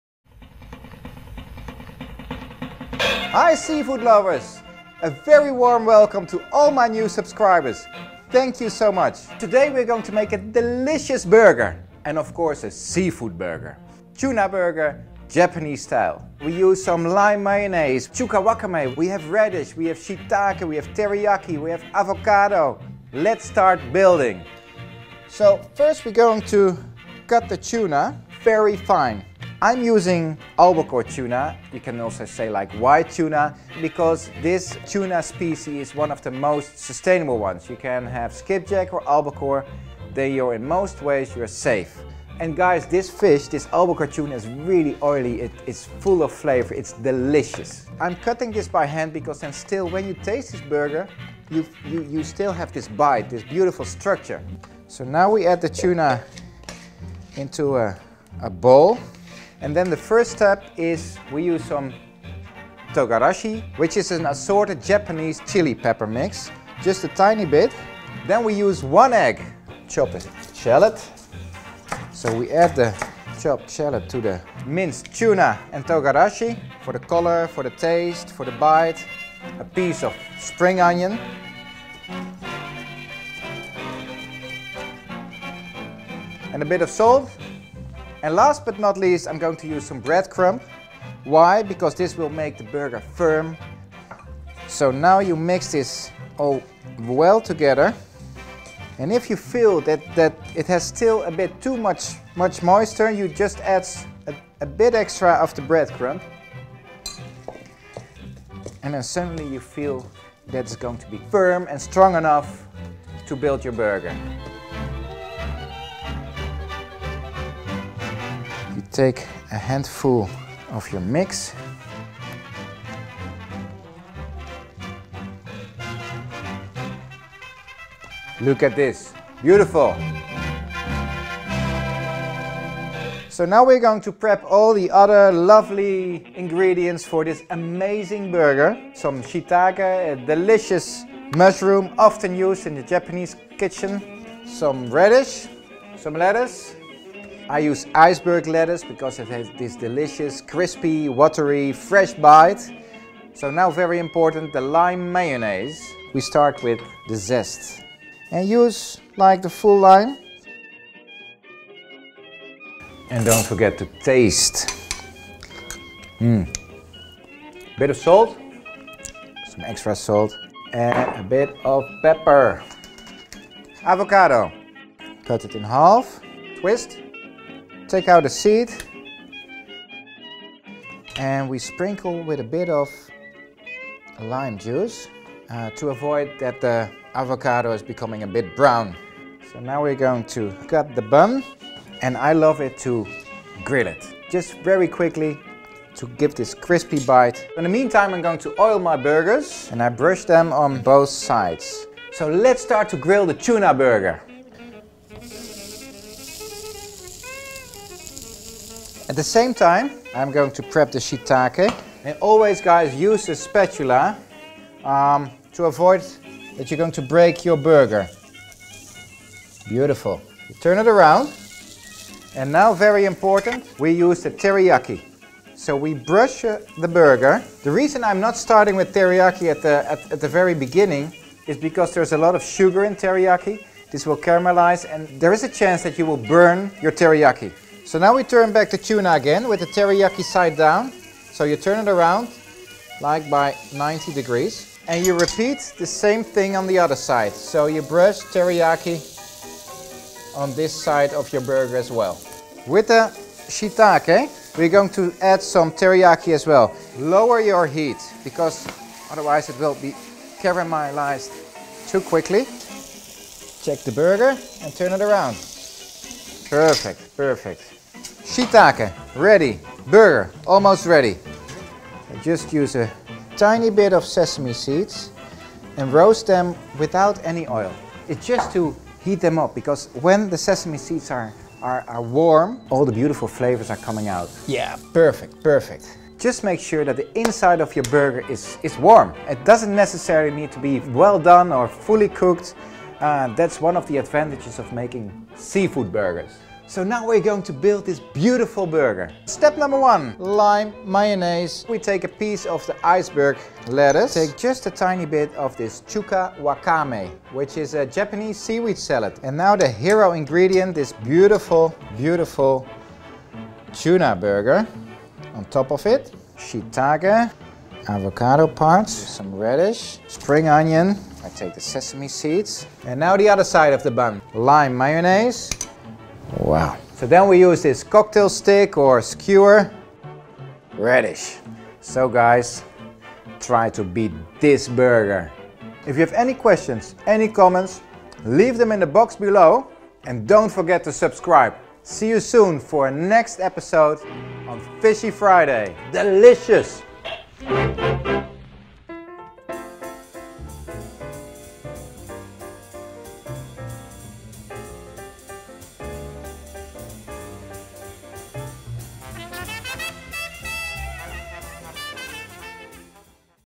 Bart's Japanese TUNA Burger Recipe | Bart van Olphen
Hi guys, welcome to a fresh Fish Tale! This time not from an outdoor spot, but from the comfort of my very own Amsterdam kitchen!

So here's my entry for the Sorted Food Beatmyburger competition: a delicious TUNA burger!

You can use almost any fish you like. Just experiment with different seafood like salmon, cod, haddock, scallops or, yes, even oysters!
Now, today, let’s start making this amazing Japanese style tuna burger! A pretty funky and exotic one with shiitake and Japanese seaweed. And believe me, it tastes damn good!
I use Albacore tuna, often called white tuna. If you choose a MSC certified tuna steak, you can be assured of working with a certified sustainable piece of fish.

See you next fishy Friday!

Video by Bart van Olphen